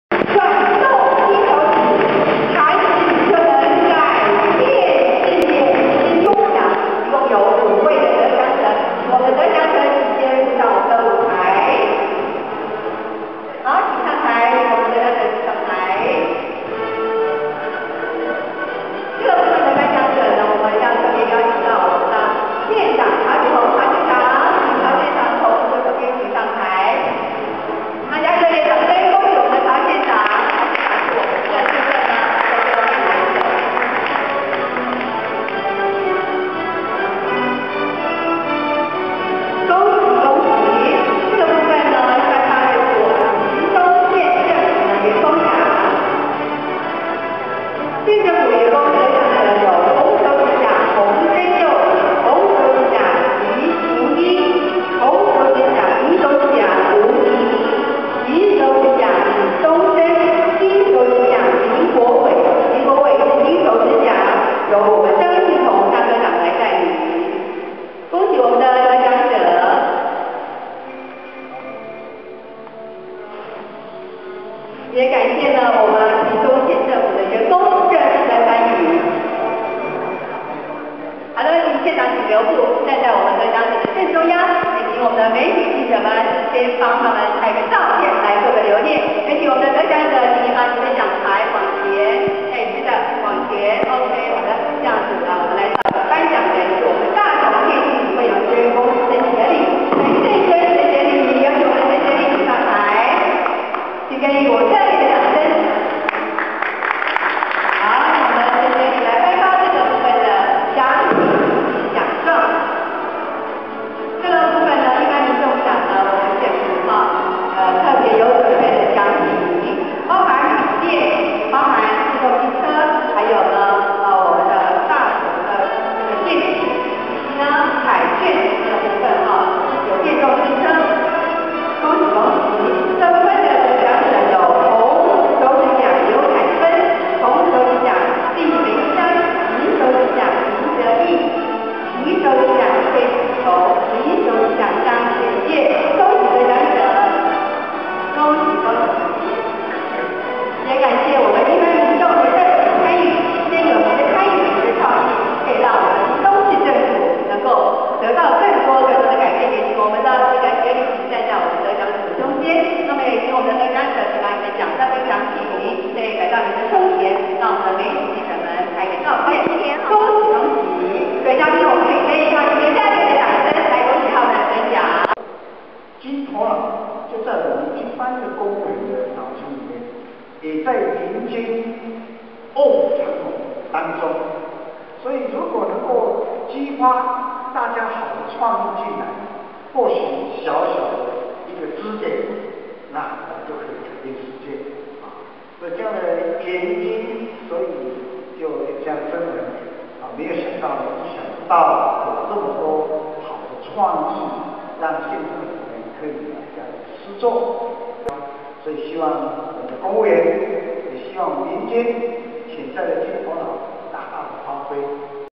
20120531屏東縣金手指頒獎表揚
20120531屏東縣金手指頒獎表揚
屏東縣政府去年首次舉辦 金手指-廣徵全民創意活動，鼓勵民眾參與公共政策議題， 獲得熱烈迴響，今年再度針對「 如何建構不動產交易價格資訊透明化機制」 及「 如何強化企業關懷社會」 兩大主題， 廣發英雄帖 ，縣長曹啟鴻31日出席活動記者會，強調民間人才臥虎藏龍，歡迎大家參與、關心切身議題，提出更多創意巧思及建言， 作為行政單位的施政參考 。